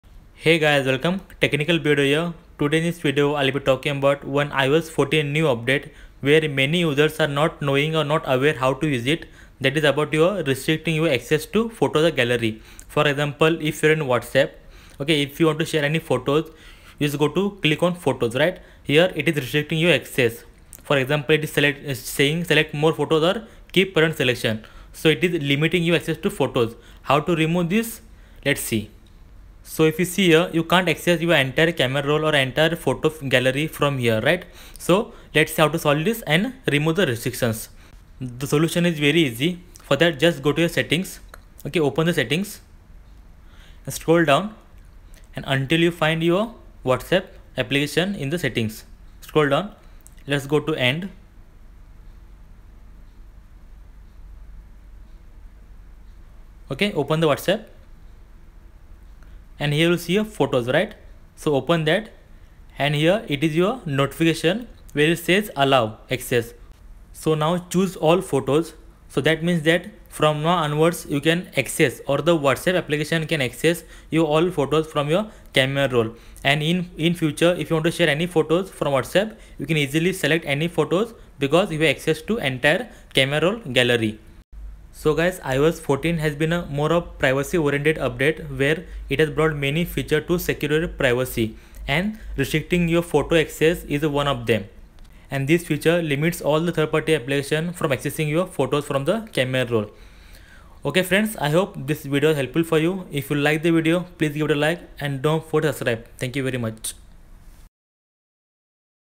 WhatsApp Only ONE photo is shown on iPhone (iOS 14) I WhatsApp would like to access your photos
After iOS 14 updates there are a lot of features added about which many user are unaware. One of them is restricting access to photos or videos from any third party apps on iPhone.
In this video watch "Whatsapp would like to access your photos" issue fixed.
How to solve only one or no photos shown in whatsapp while sharing photos.
Whatsapp cannot access photos or phone gallery while sharing photos in iphone.
Only one image shown in whatsapp while sharing media after iPhone ,iOS 14 update.
iOS 14 update stopped photos access in whatsapp app in iPhone.